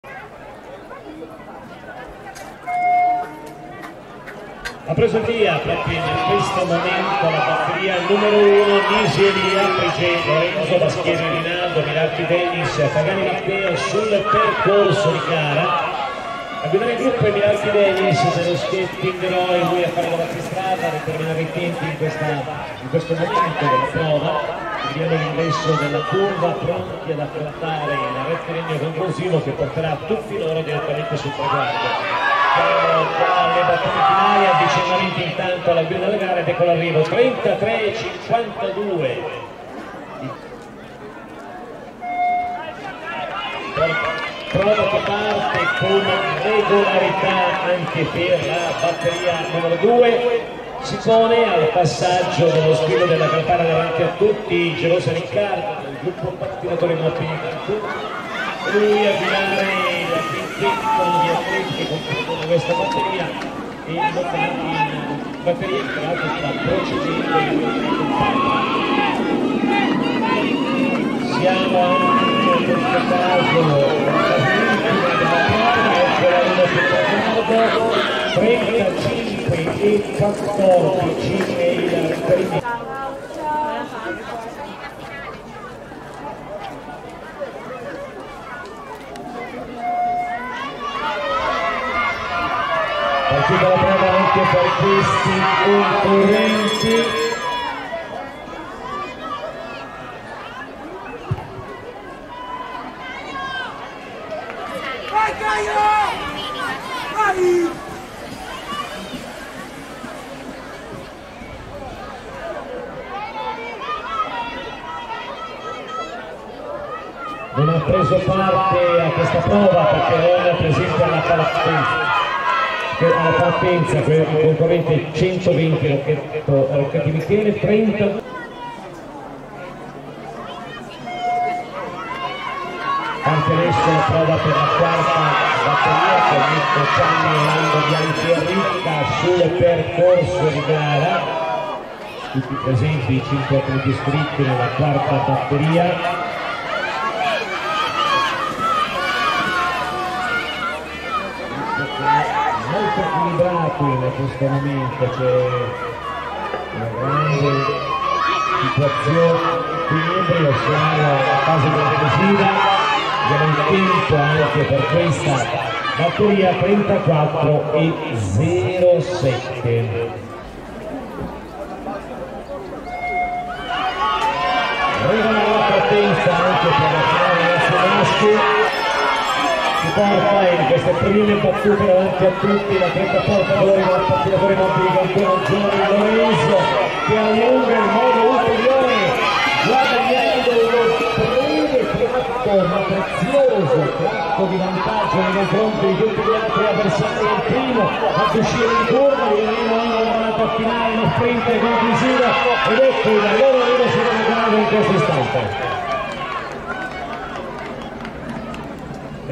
XV Trofeo di Primavera 2022 - R/M 1 giro sprint, qual e finale
00:00 qualifiche, batteria 1
00:40 qualifiche, batteria 2
01:23 qualifiche, batteria 3
02:08 qualifiche, batteria 4
02:50 Finale

Pattinaggio velocità su rotelle, specialità strada (speed skating on wheels, road)
XV Trofeo di Primavera 2022
18 Aprile 2022, Paderno d'Adda (LC) - Italy
Categoria Ragazzi Maschile
1 giro sprint, qual e finale

1  56  BASCHIERI RINALDO  589  SKATING C.CASSANO D.  33.487
2  200  FRIGE' LORENZO  3816  REDBLACK R.TEAM  33.692
3  231  MIRARCHI DENNIS  1290  SKATING RHO  33.764
4  343  NISI ELIA  4333  R.S.VADO LIGURE ASD  34.288
5  46  PAGANI MATTEO  651  G.P.MOBILI CANTU'  36.053

1  18  CAROLEO LORENZO  310  POL. BELLUSCO  35.125
2  44  GEROSA RICCARDO  651  G.P.MOBILI CANTU'  35.219
3  218  SITNIC CRISTIANO  1285  PATT.S.MAURO TO  35.491
4  50  ZAPPATERRA SIMONE  3816  REDBLACK R.TEAM  35.573
5  328  BAGNOLINI FILIPPO  2296  PATT.RI BONONIA  37.724

1  170  MAGGIONI LORENZO  651  G.P.MOBILI CANTU'  32.333
2  57  GIANA MATTEO  561  ANSPI POL.S.NAZARIO  32.512
3  230  CAIOLO ANDREA  1290  SKATING RHO  34.264
4  75  SIVIGLIA ALESSANDRO  3814  ANPINLINE  36.166
 120  ROCCHETTI MICHELE  4021  BI ROLLER  NP

Ammonizione al n. 57 GIANA MATTEO

1  211  RICCA MATTIA  3814  ANPINLINE  34.080
2  286  COGNETTO ANGELO  1290  SKATING RHO  34.826
3  19  CIANNI FEDERICO  310  POL. BELLUSCO  34.981
4  252  ROLANDO MATTEO  1002  VITTORIA TORINO  35.103
5  201  BIANCHI ANDREA  1136  SKATING C.UBOLDO  37.329

Risultati batteria n.5 (video mancante)
1  149  TAGLIABUE CARLO  3809  A.S.D.BRIANZA INLINE  34.583
2  72  FIORINI GIOVANNI  310  POL. BELLUSCO  34.654
3  48  NICOLINI ALESSANDRO  3816  REDBLACK R.TEAM  35.506
4  77  SIVIGLIA FILIPPO  3814  ANPINLINE  35.571
 92  MAZZOLA ANDREA  4021  BI ROLLER  NP

Risultati
1  170  MAGGIONI LORENZO  651  G.P.MOBILI CANTU'  31.866
2  57  GIANA MATTEO  561  ANSPI POL.S.NAZARIO  32.634
3  200  FRIGE' LORENZO  3816  REDBLACK R.TEAM  32.968
4  211  RICCA MATTIA  3814  ANPINLINE  33.581
4  231  MIRARCHI DENNIS  1290  SKATING RHO  33.581
6  56  BASCHIERI RINALDO  589  SKATING C.CASSANO D.  33.584